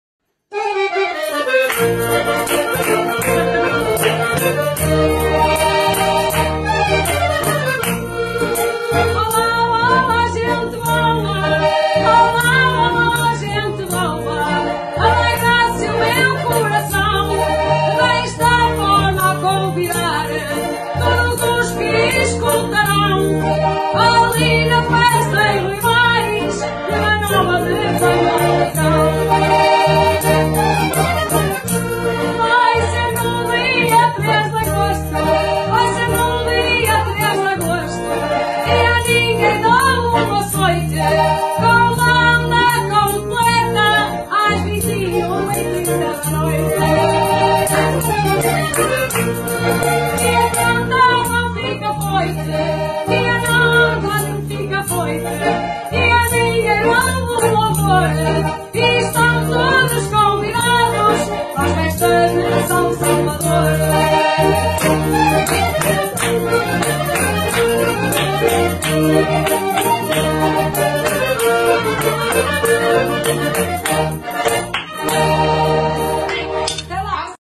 Liliana Oliveira com Coração Minhoto à desgarrada.
Cantares ao desafio. Convite para espetáculo na Suiça.